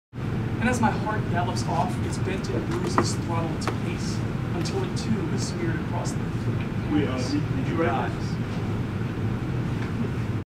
angry Sven is angry
The Sven-Bo! gives a heckler the death stare.